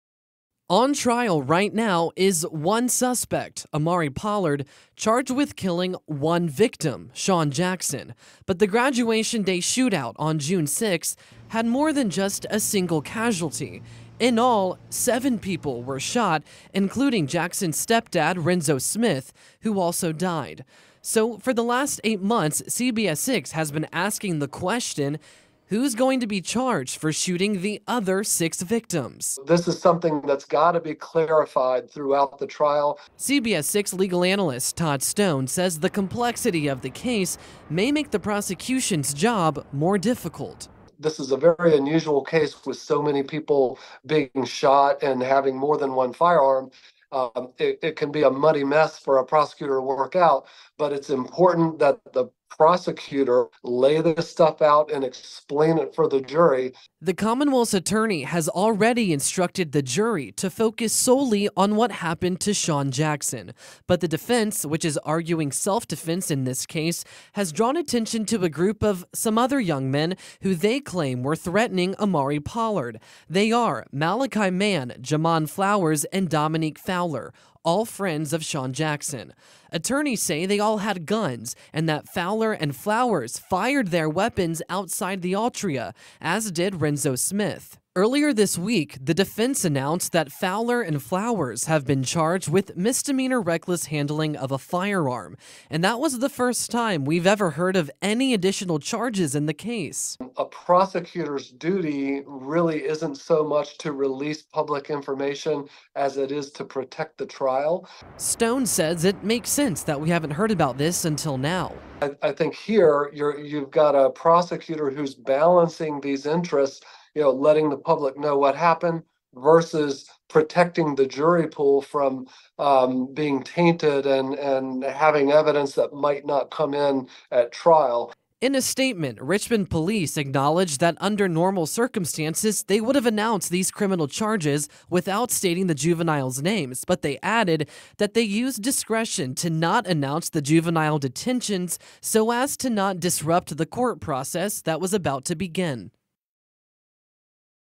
Who else is charged in Graduation Day shootings? I'm finally getting answers.
For the first time since the deadly June 6 shooting, new information was made public that shed light on other individuals who have been charged in connection to the shooting.